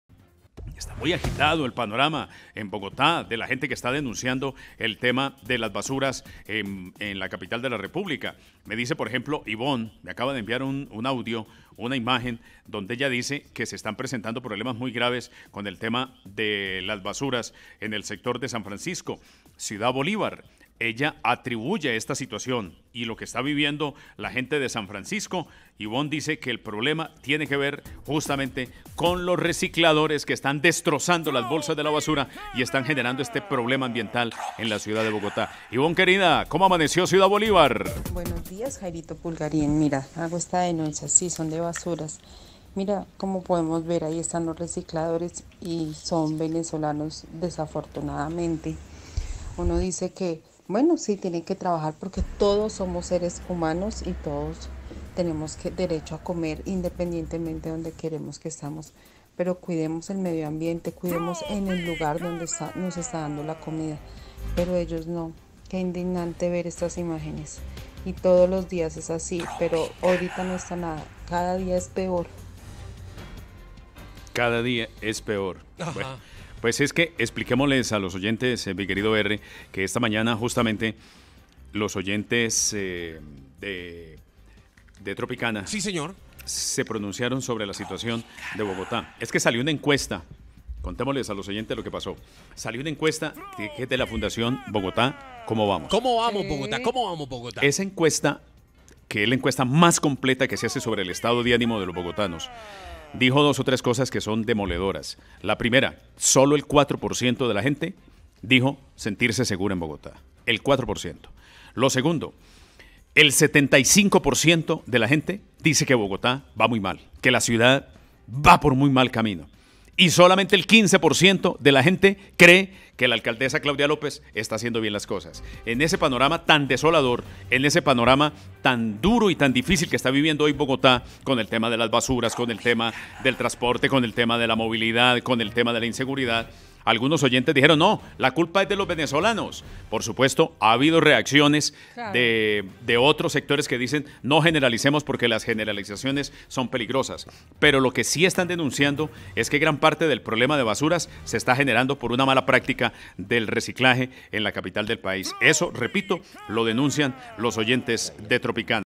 Graves problemas de basura en Bogotá
Ciudadanos denuncian problemas de basura en Ciudad Bolivar debido a los recicladores que rompen las bolsas de basura.